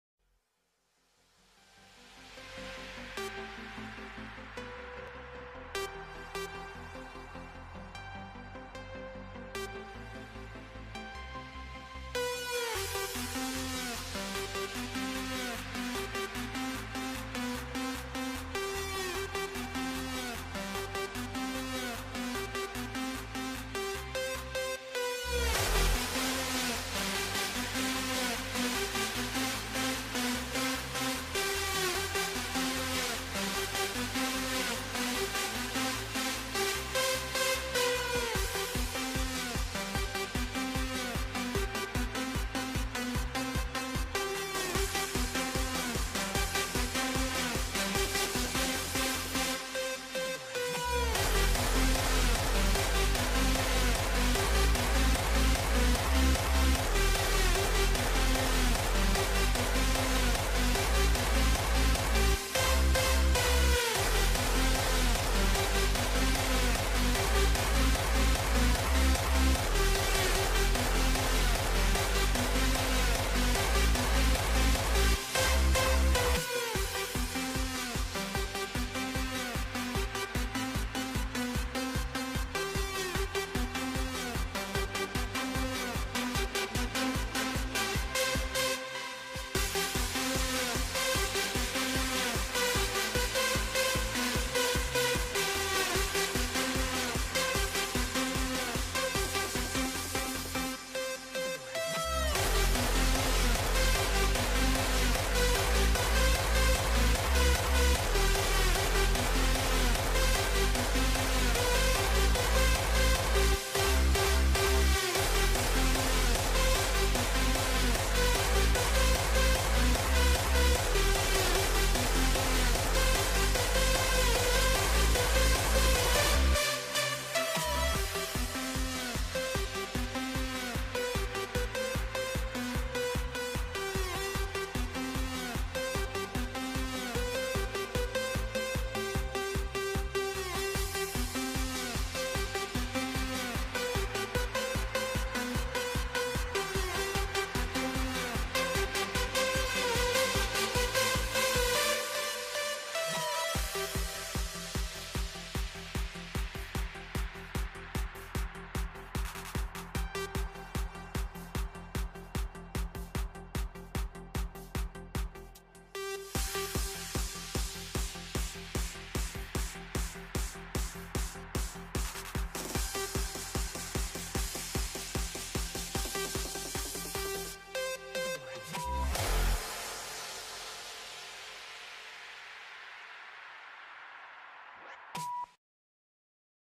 Ocularis Inferno take down 2 caps in a C4!
Ocularis Inferno stumble upon a fight and shortly find themselves against a bigger foe: two caps in a C4. Can they take them? I think so.